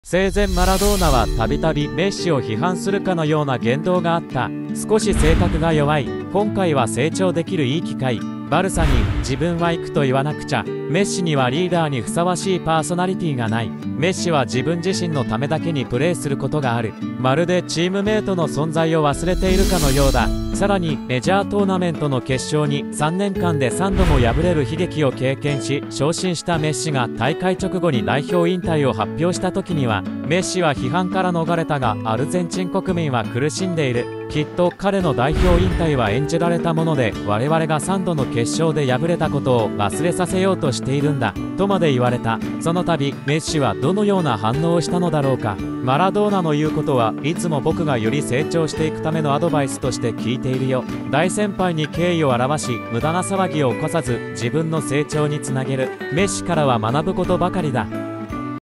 #3.メッシから学ぶ人生哲学マラドーナからの批判に対するメッシの反応
リオネルメッシから学ぶ人生哲学
英雄マラドーナからの批判
更にメジャートーナメントの決勝に3年間で3度も敗れる悲劇を経験し傷心したメッシが大会直後に代表引退を発表した時には。
「メッシは批判から逃れたがアルゼンチン国民は苦しんでいる。きっと彼の代表引退は演じられたもので我々が3度の決勝で敗れたことを忘れさせようとしているんだ」
とまで言われた。
その度メッシはどの様な反応をしたのだろうか。

マラドーナの言うことはいつも僕がより成長していくためのアドバイスとして聞いているよ。

大先輩に敬意を表し無駄な騒ぎを起こさず自分の成長に繋げる。
メッシからは学ぶ事ばかりだ...